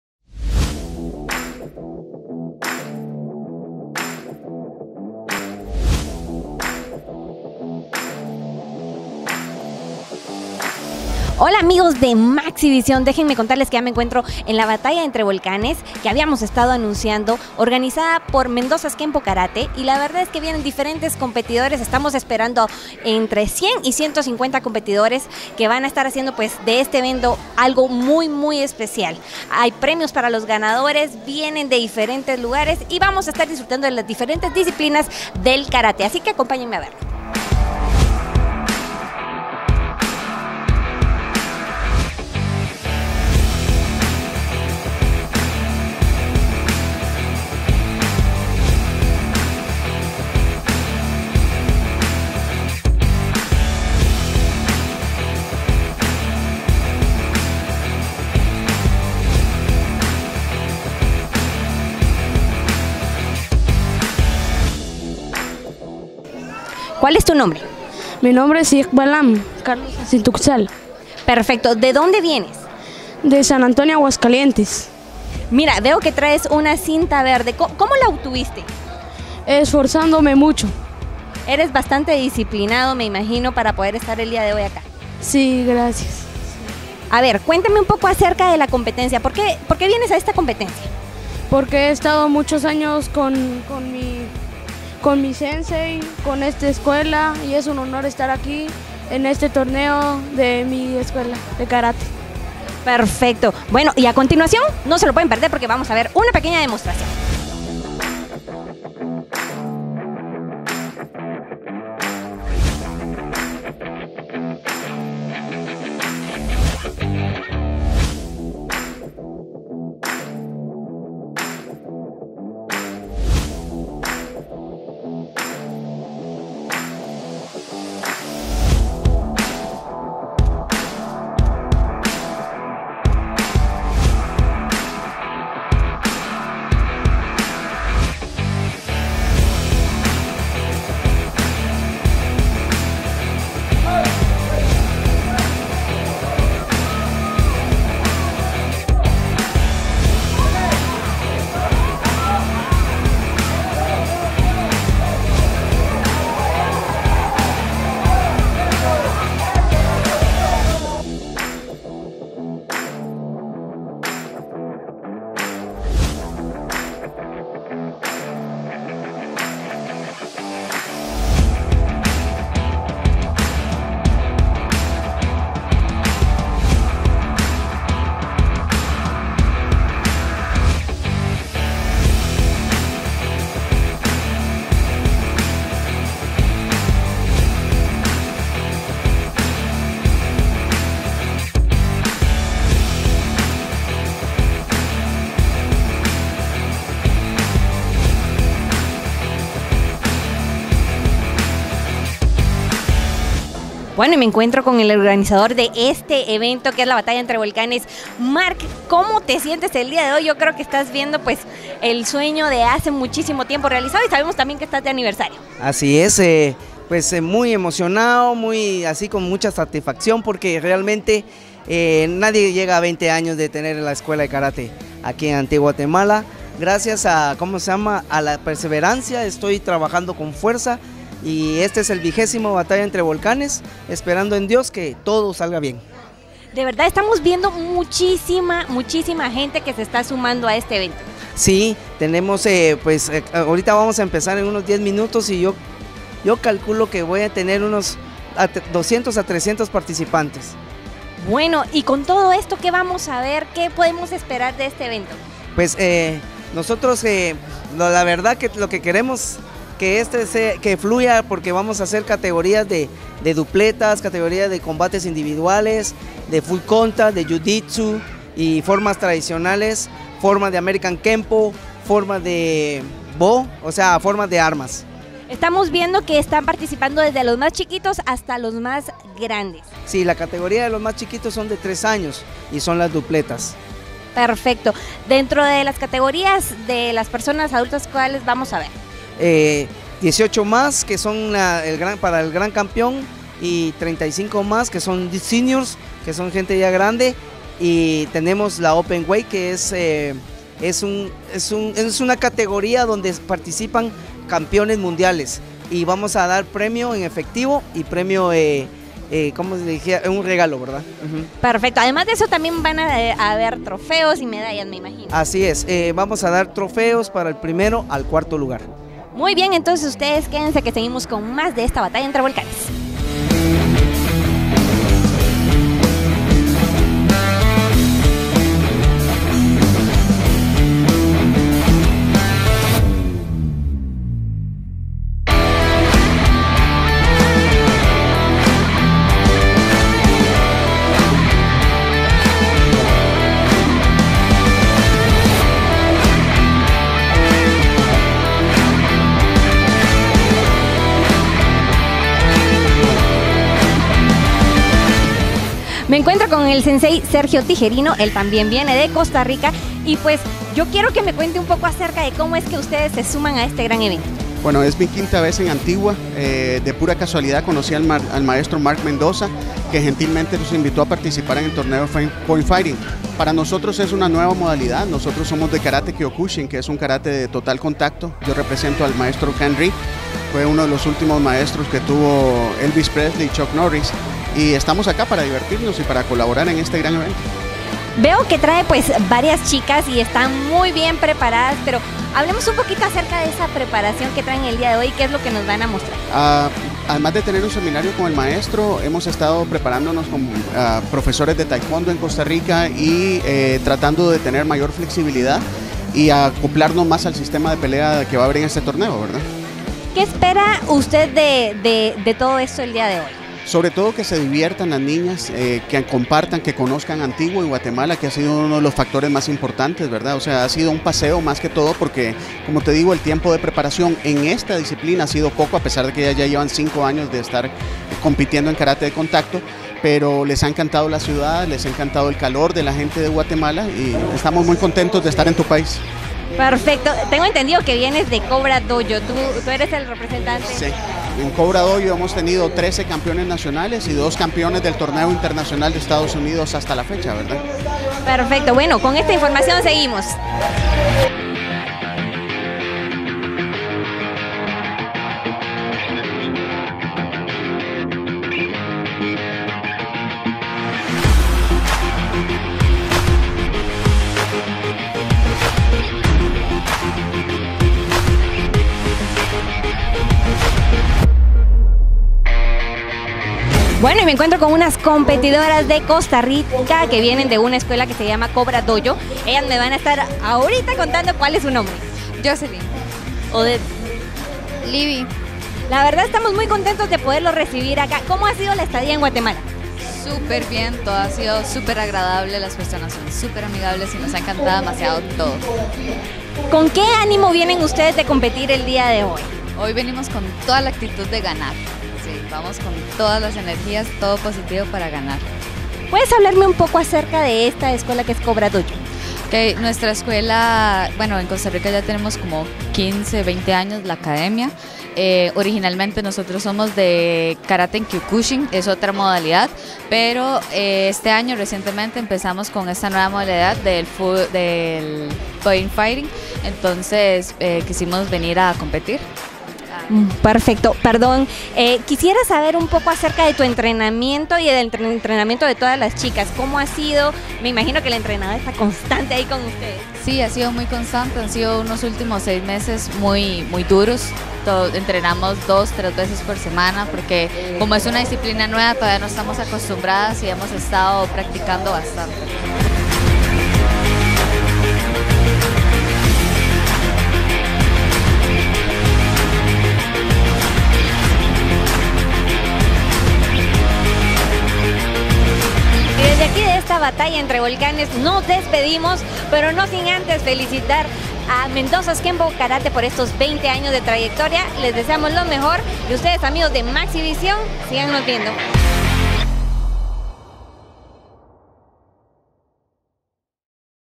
Batalla Entre Volcanes MAXISPORTS
Te traemos un resumen de lo que vivimos el domingo 28 de julio en Batalla Entre Volcanes 2019 donde las artes marciales fueron las protagonistas!

MaxiVisión, Lo que quieres ver!

Maxivisión Tv